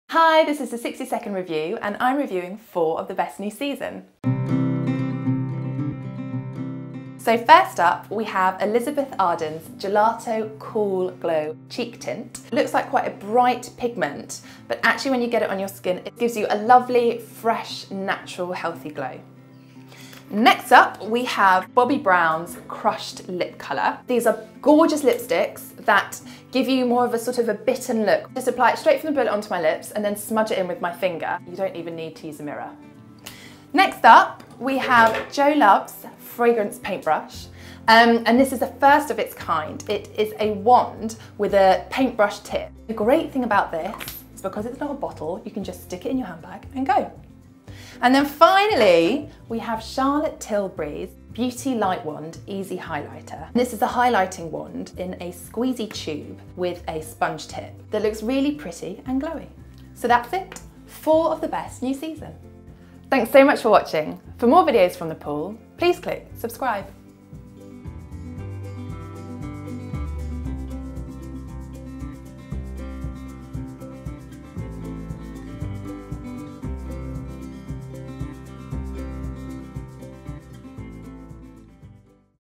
The best new season beauty buys | 60 Second Reviewer | The Pool
Charlotte Tilbury's new highlighter, perfume in a paintbrush and a lovely new lipstick – Frankie Graddon reviews the best of the new season's beauty buys

ABOUT THE POOL:
The Pool is the home of original content for women too busy to browse.